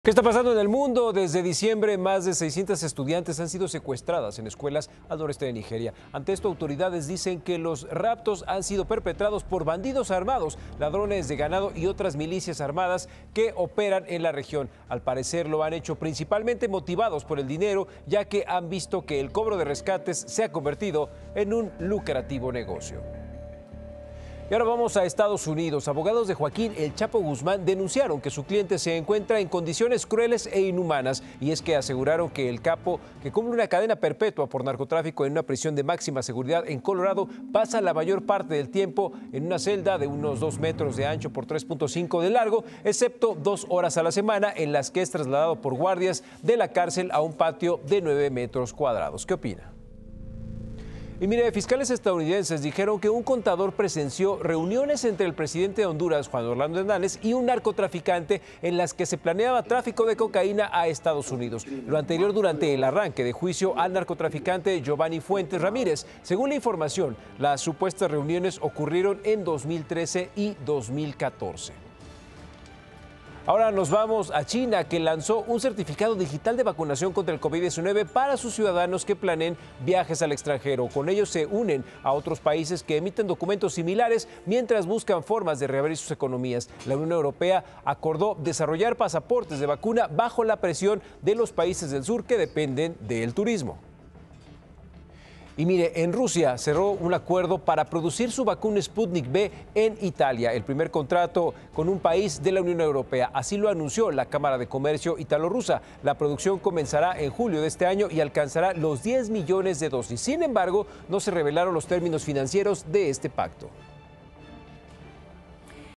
Al menos 600 estudiantes han sido secuestrados en escuelas de Nigeria desde diciembre | Paco Zea
Desde diciembre más de 600 estudiantes han sido secuestradas en escuelas al noreste de Nigeria. Autoridades dicen que los raptos han sido perpetrados por bandidos armados que operan en la región.